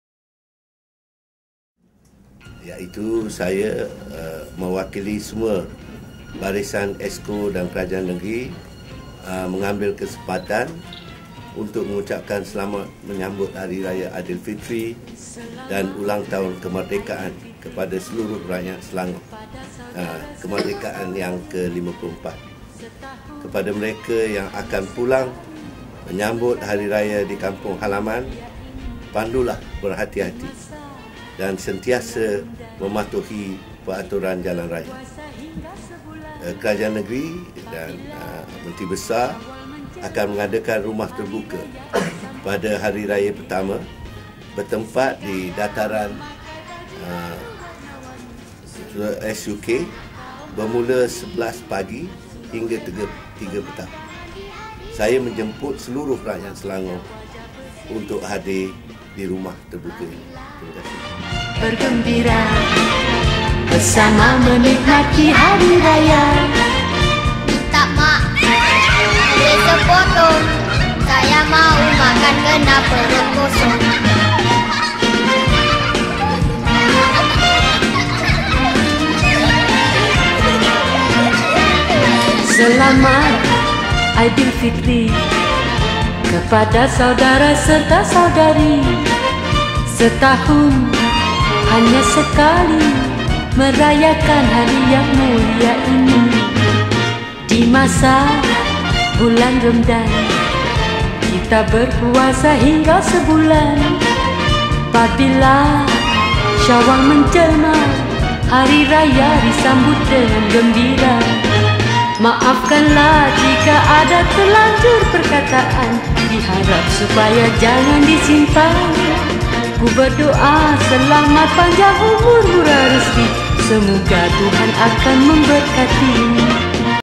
Jemputan Rumah Terbuka Hari Raya 2011.wmv
Jemputan Rumah Terbuka Hari Raya YAB Dato' Menteri Besar Selangor 2011
Semua Dijemput Hadir.